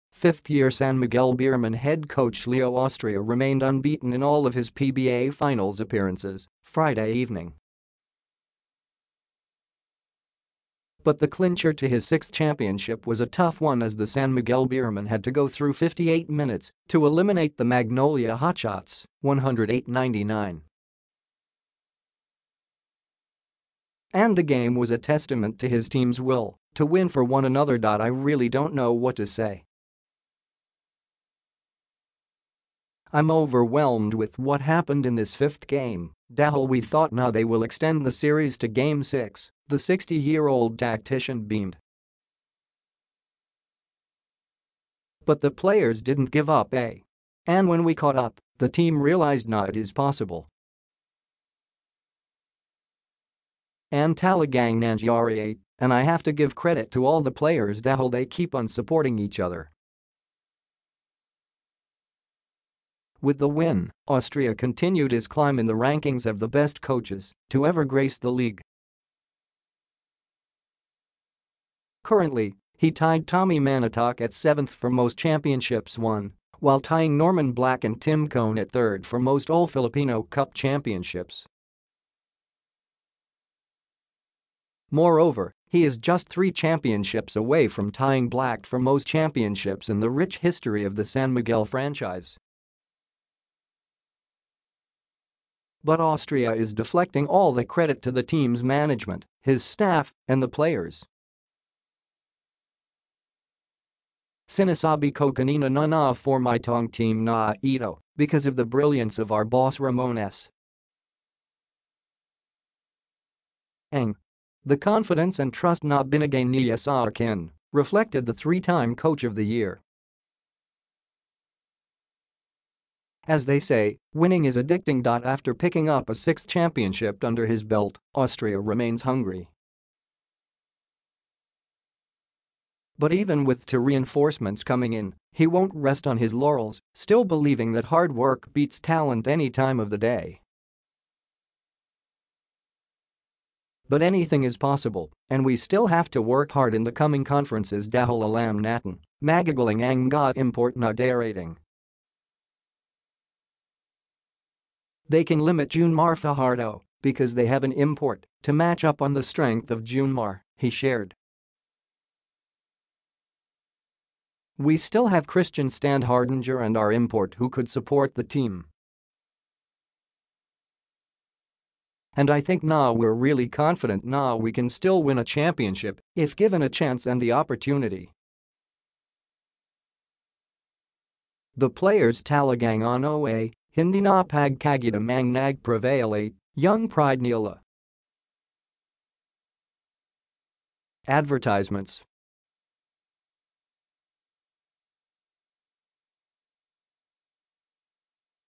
Leo Austria does not have any plans on losing in the Finals: ‘I’m planning to extend it’
Fifth-year San Miguel Beermen head coach Leo Austria remained unbeaten in all of his PBA Finals appearances, Friday evening.But the clincher to his sixth championship was a tough one as the San Miguel Beermen had to go through 58 minutes to eliminate the Magnolia Hotshots, 108-99. And the game was a testament to his team’s will to win for one another.“I really don’t know what to say. I’m overwhelmed with what happened in this fifth game, dahil we thought na they will extend the series to Game Six,” the 60-year-old tactician beamed. “But the players didn’t give up eh. And when we caught up, the team realized na it is possible. And talagang nangyari eh, and I have to give credit to all the players dahil they keep on supporting each other.With the win, Austria continued his climb in the rankings of the best coaches to ever grace the league. Currently, he tied Tommy Manotoc at seventh for most championships won, while tying Norman Black and Tim Cone at third for most All-Filipino Cup championships. Moreover, he is just three championships away from tying Black for most championships in the rich history of the San Miguel franchise.But Austria is deflecting all the credit to the team’s management, his staff, and the players.“Sinasabi ko kanina na na-form itong team na ito because of the brilliance of our boss Ramon S. Ang. The confidence and trust na binigay niya sa akin,” reflected the three-time Coach of the Year.As they say, winning is addicting.After picking up a sixth championship under his belt, Austria remains hungry. But even with “two reinforcements” coming in, he won’t rest on his laurels, still believing that hard work beats talent any time of the day.“But anything is possible, and we still have to work hard in the coming conferences dahil alam natin, magagaling ang mga import na darating. They can limit June Mar Fajardo, because they have an import to match up on the strength of June Mar,” he shared. “We still have Christian [Standhardinger] and our import who could support the team. And I think na we’re really confident na we can still win a championship if given a chance and the opportunity. The players talagang ano eh, hindi na pagkagutom ang nagpe-prevail eh — yung pride nila.” Advertisements